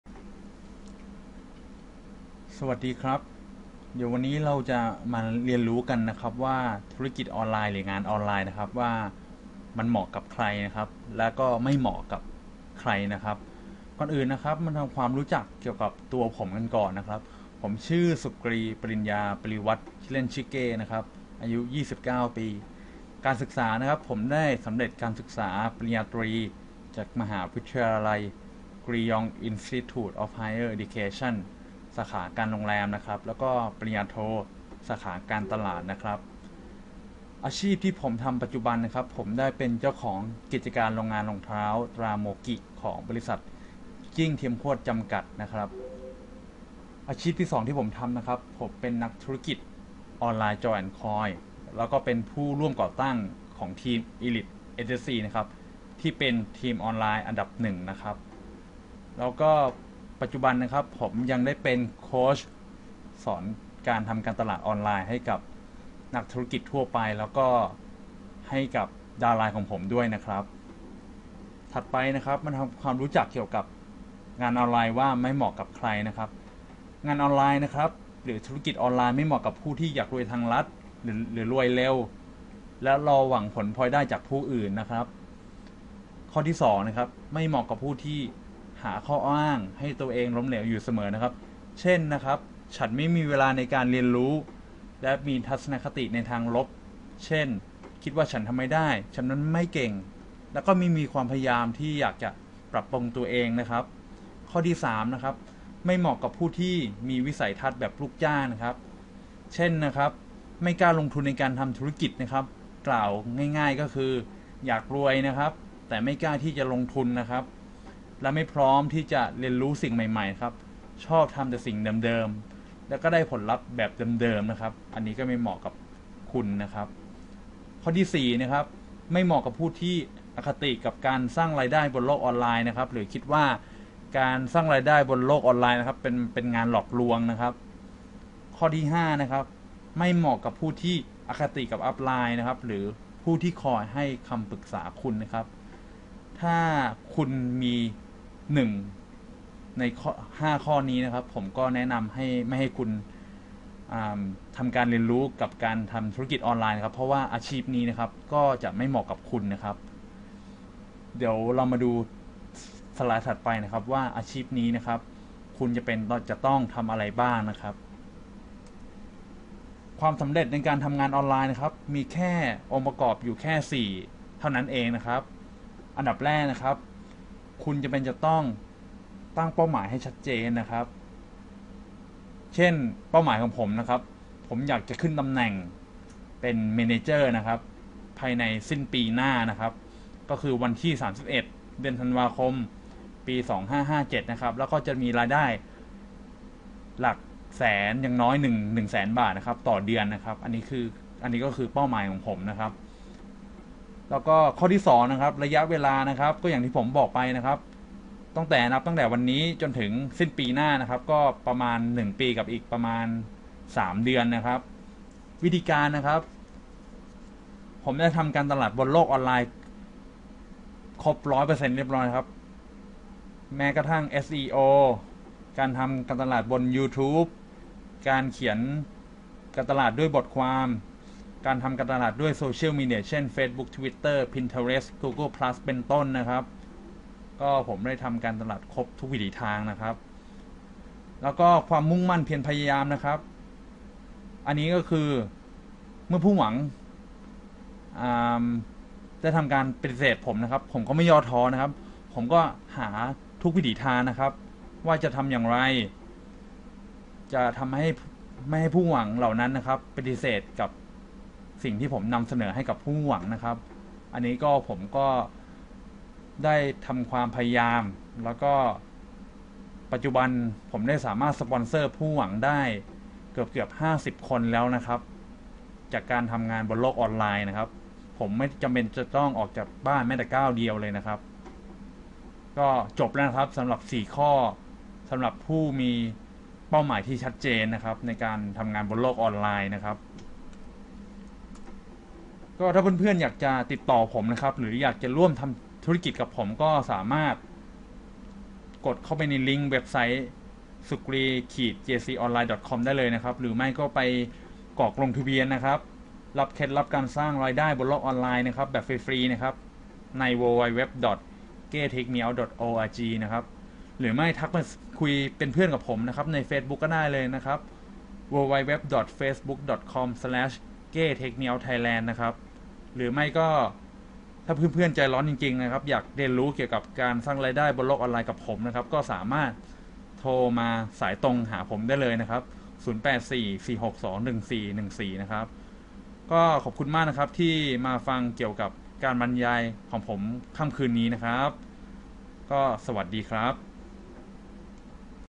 ธุรกิจออนไลน์ เหมาะกับใครและไม่เหมาะกับใคร โดย สุกรี ปริญญาปริัวัฒน์
ศึกษาข้อมูลเพิ่มเติมเกี่ยวกับการทำธุรกิจออนไลน์ได้ที่
ทักมาคุยที่เฟสผมได้ที่
ติดต่อผมโดยตรงได้ที่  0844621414 สุกรี (เก้) ปริญญาปริวัฒน์